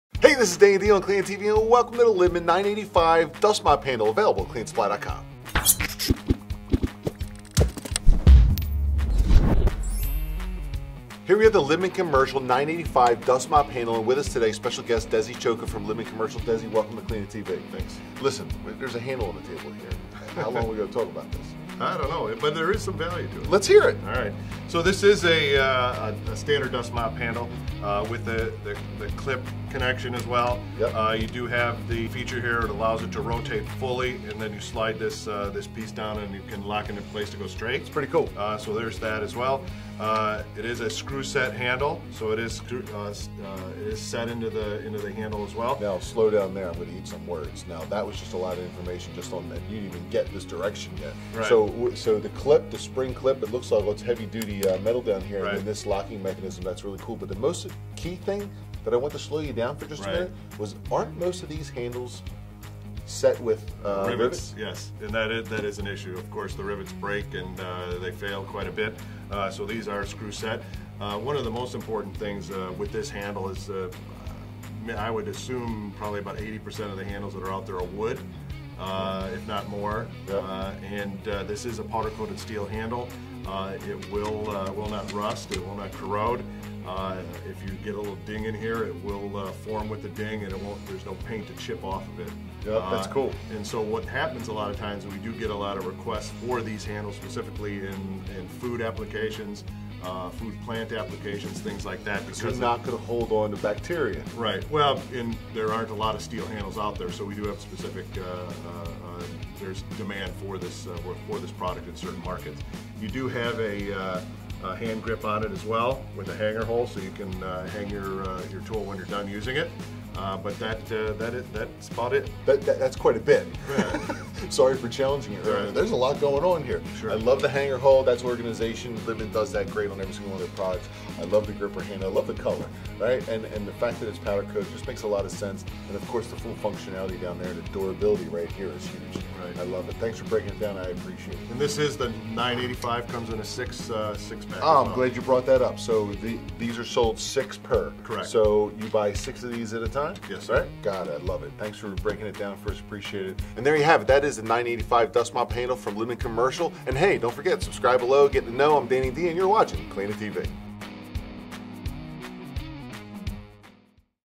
Libman Dust Mop 61" Handle
Libman Dust Mop Handles

Libman dust mop handles are chrome plated, with a steel clip on a swivel mop attachment. The swivel makes getting around furniture quick and easy. This 1" sturdy steel handle has a 5" grip with hanger tip for easy storing. Total length 61". Lead time for orders is 2-3 Business Days.

Libman Dust Mop Handles, 6 Handles per Case -